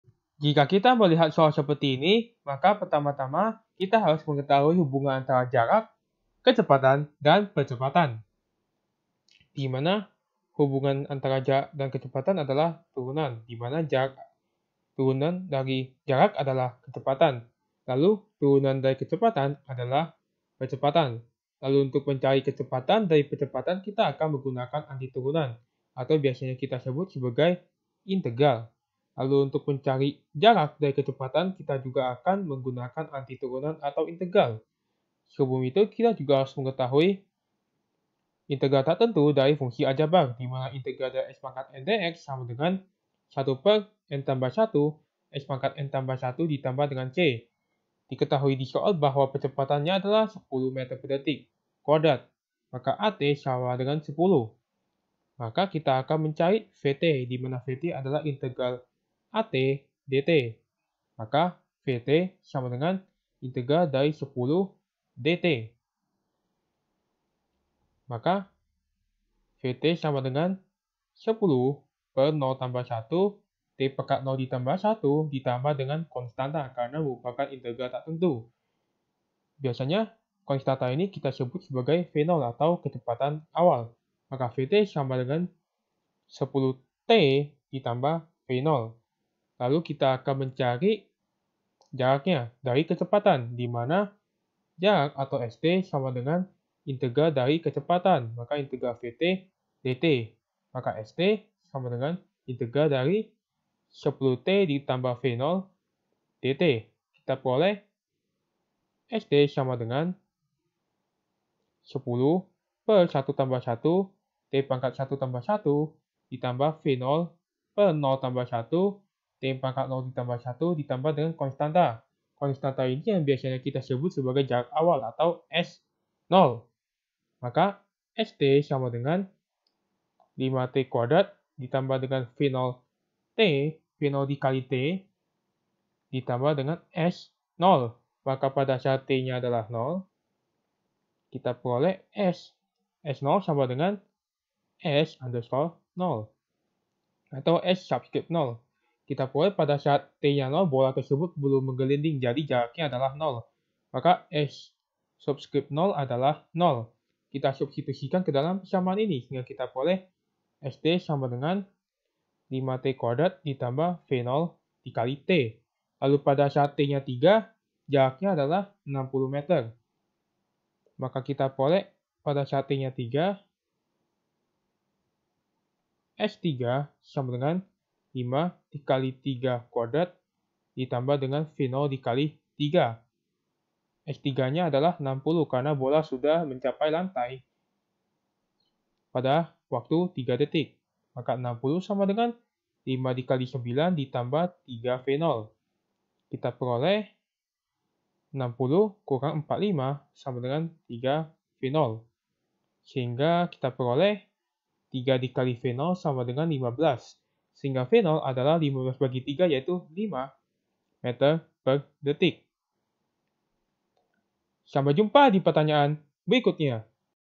Sebuah bola menggelinding pada bidang miring dengan percepatan 10 meter/detik?. Jika panjang suat...
Sebuah bola menggelinding pada bidang miring dengan percepatan 10 meter/detik?. Jika panjang suatu bidang miring tersebut 60 meter dan bola mencapai lantai dalam waktu 3 detik, tentukan kecepatan awal bola

Topik atau Materi: Integral Tentu - Integral Tentu - KALKULUS - Matematika Kelas 11 SMA - K-2006

Sumber Soal: Matematika SMA IPA kelas 12 KTSP, MSG 2008 , Quadra

00:00 : Bedah Soal
01:00 : Konsep, rumus dan pengertian Integral Tentu
01:18 : Langkah penyelesaian soal
02:37 : Jawaban akhir
03:07 : Penutup